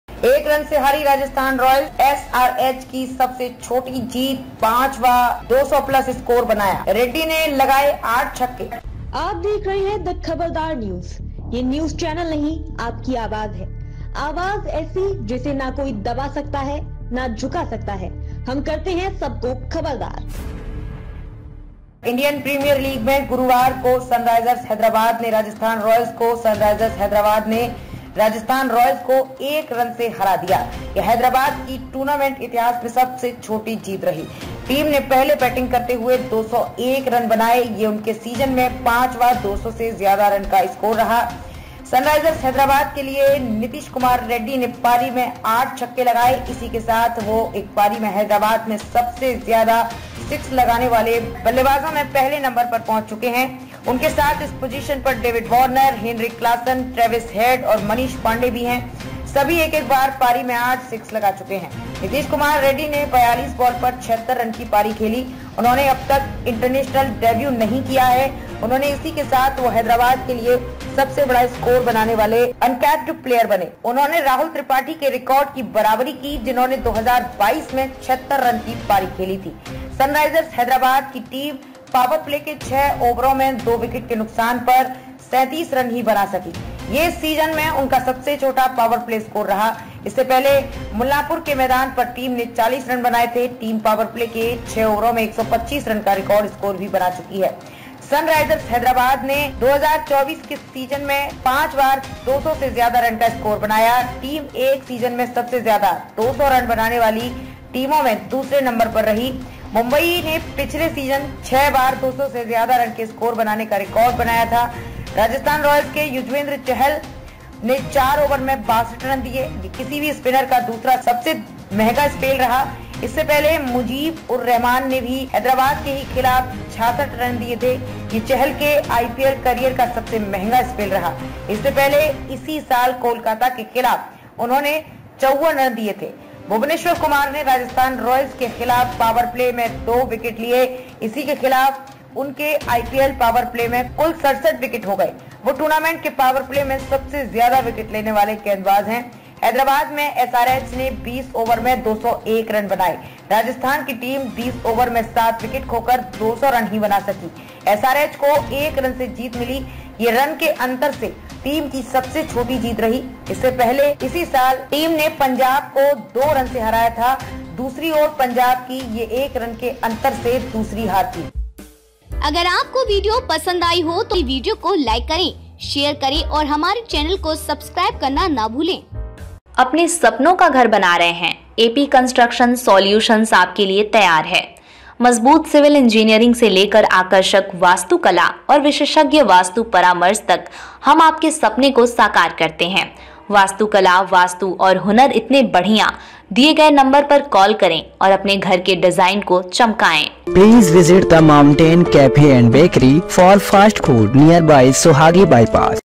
IPL 2024 Update : एक रन से हारी Rajasthan Royals SRH की सबसे छोटी जीत, 5वां 200+ स्कोर बनाया,
सनराइजर्स हैदराबाद के लिए नितिश कुमार रेड्डी ने पारी में 8 छक्के लगाए। इसी के साथ वह एक पारी में हैदराबाद से सबसे ज्यादा सिक्स लगाने वाले बैटर में पहले नंबर पर पहुंचे। उनके साथ इस पोजिशन पर डेविड वॉर्नर, हेनरिक क्लासन, ट्रैविस हेड और मनीष पांडे भी हैं। सभी एक-एक बार पारी में 8 सिक्स लगा चुके हैं।